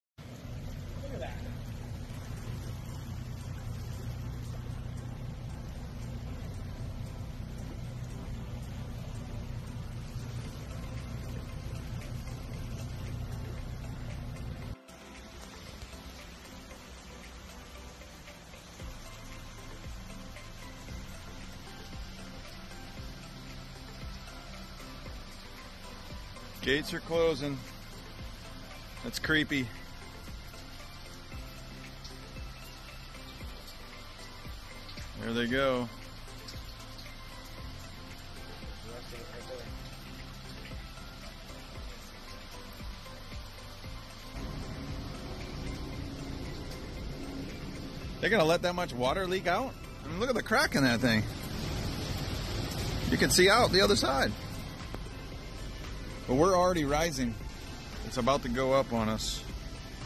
The LOCKS of the Dalles Dam!
I got to experience going through the LOCKS of the Dalles Dam in my bass boat. It was my first time and it was amazing as well as creepy!

Our Motto:
We LIVE to DREAM to FISH